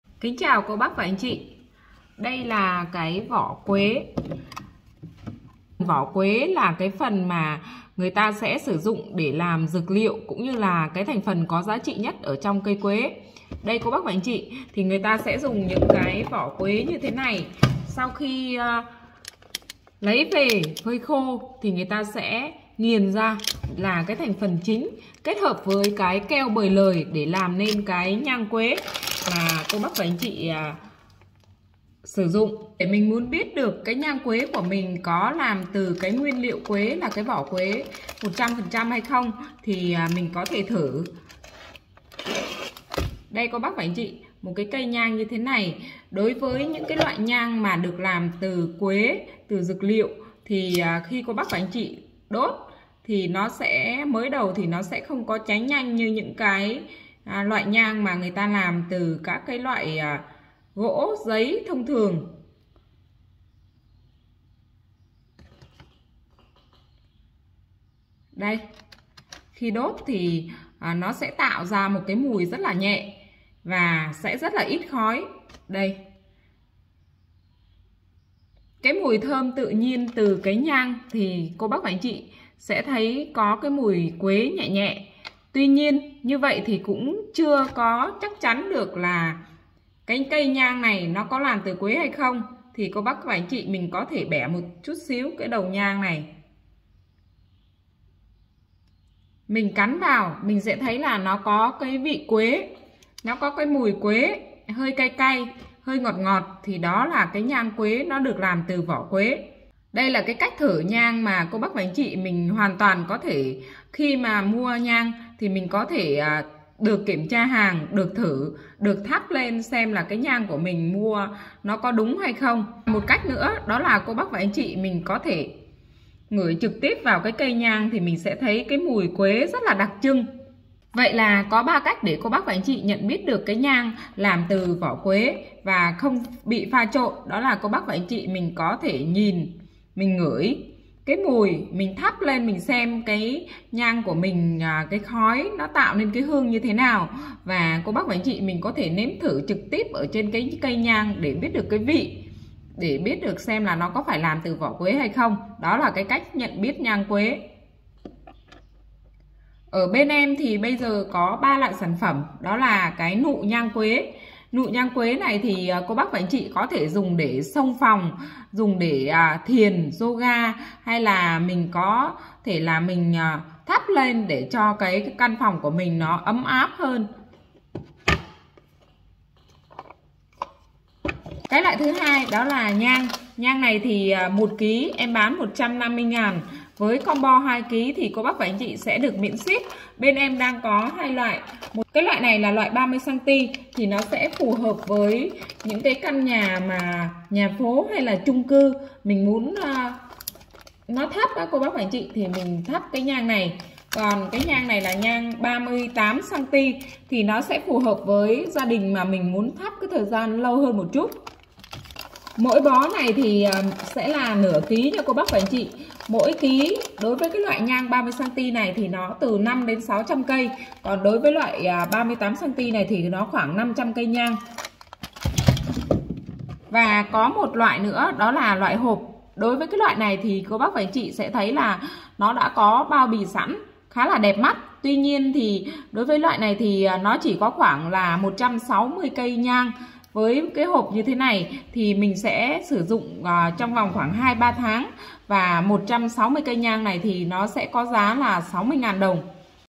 CÁCH NHẬN BIẾT NHANG QUẾ 100% CHUẨN KHÔNG BỊ NHẦM LẪN.
cách nhận biết nhang quế